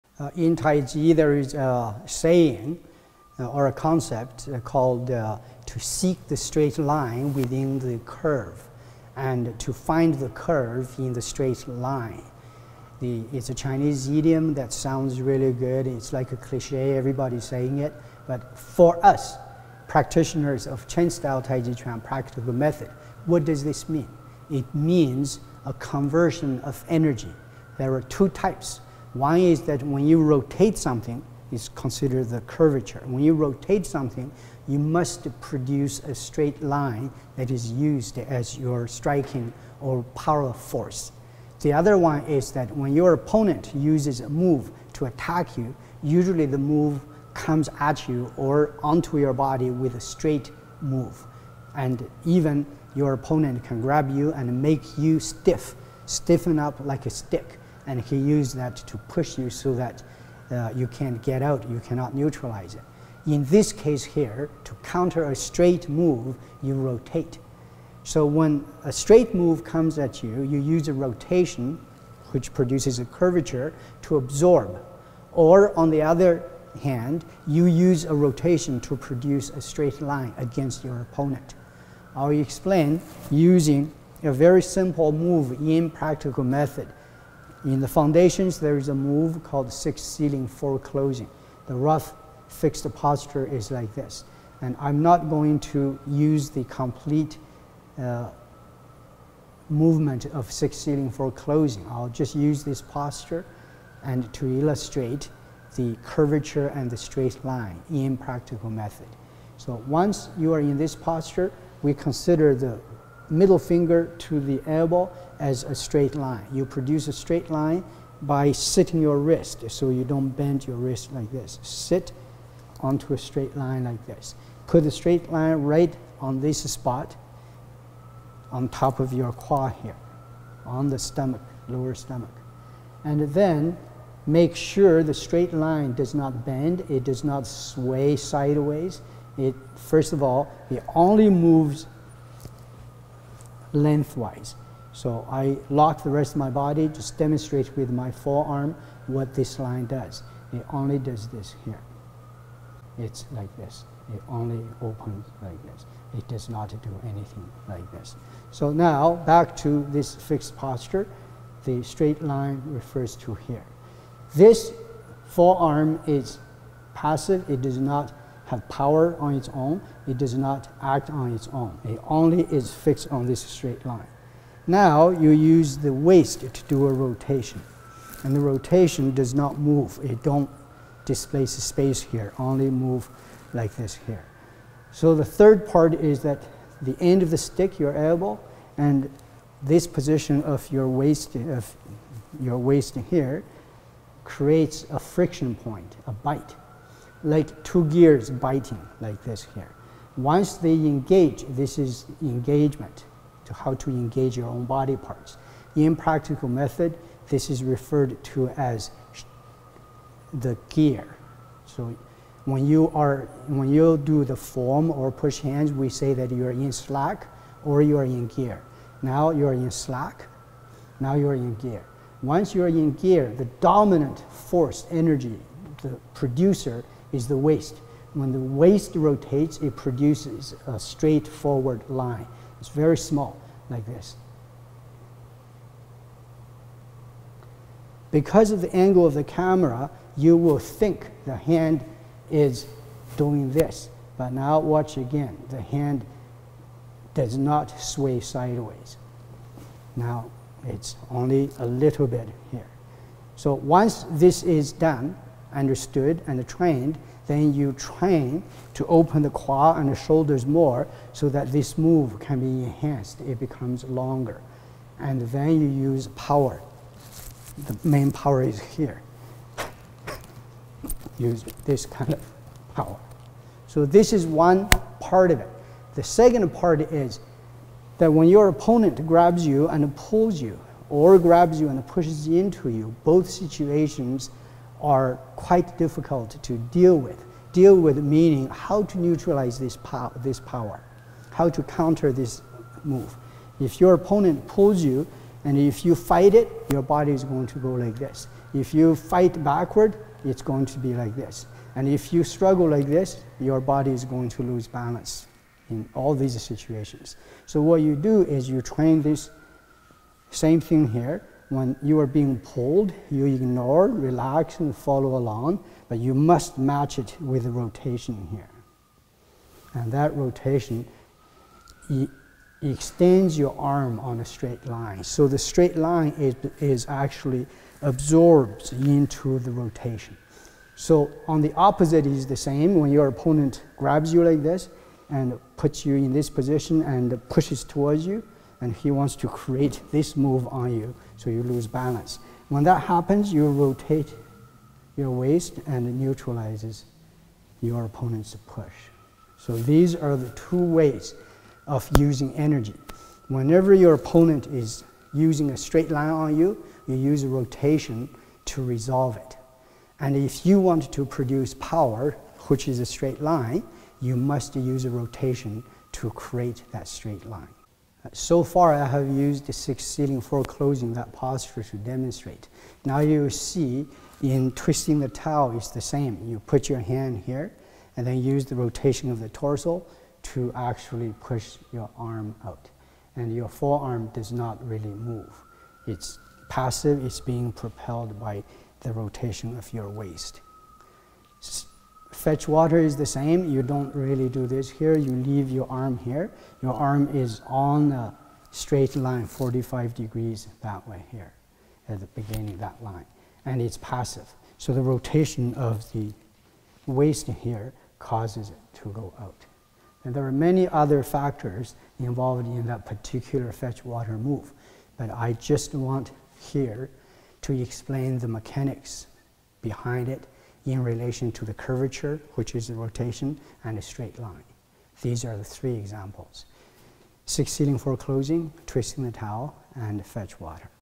Qu Zhong Qiu Zhi-Terminology
Qu (Curve), Zhong (In), Qiu (Seek), Zhi (Straight).

To seek the straight line in the curvature. This is an important concept for all taiji styles.
This mini video is Master Chen Zhonghua's definition of the concept for Chen Style Taijiquan Practical Method.